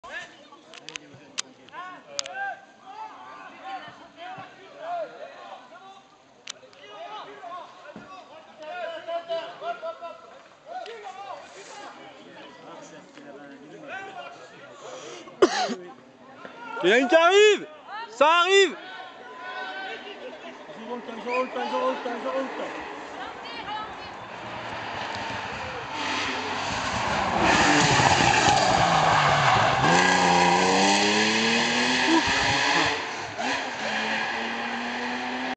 rallye des vins macon 2011 "ES2" igé verzé
vehiclue VH , sorti de route simca 1000 et porshe qui arrive pleine balle ! c'etait chaud cacao !